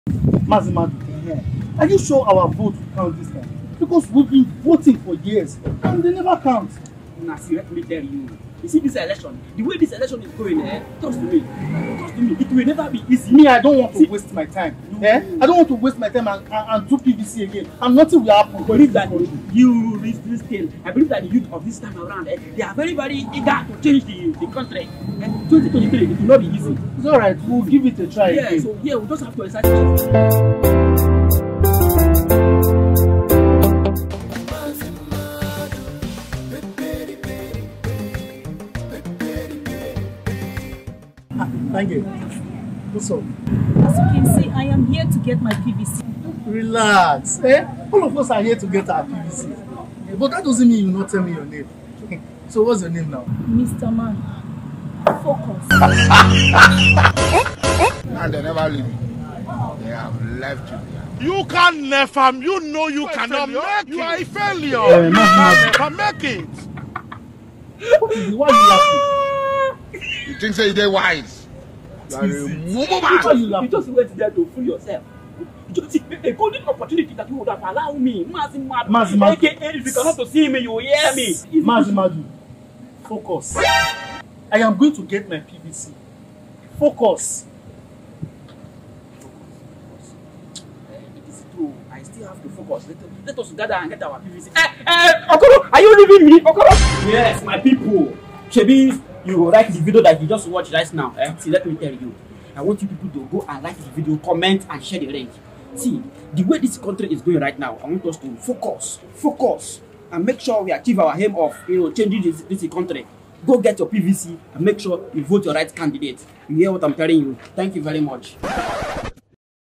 Mazi Madu focus, PVC wahala. pvc election #2023
Mazi Madu and Okoro his friend at a PVC center as Okoro try to shoot his shot at a beautiful damsel while she instructed him to focus on getting his  PVC. This short video shows the interest and seriousness of the general public in getting their PVC.